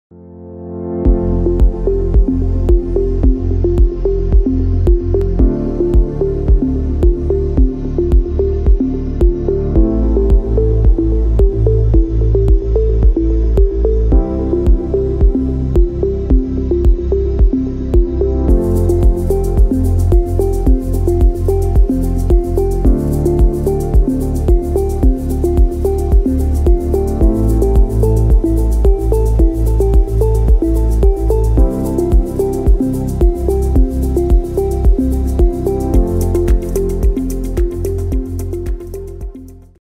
Seifensender Schaumseife Manuell Blanc Hygienic COSMOS 1250 1080p
Schaumseifenspender, Seifenspender
Der innovative und absolut ergiebige Cosmos Schaumseifenspender von Blanc Hygienic

Zahlreiche Mehrwerte vereint in einem formschönen Seifenspender:

- enorme Ergiebigkeit, 2.500 Anwendungen je Kartusche
- sekundenschneller Kartuschenwechsel
- abschließbar & sicher
- Kartuschen mit neuer vormontierter Pumpe
- einfachste Montage
- uvm.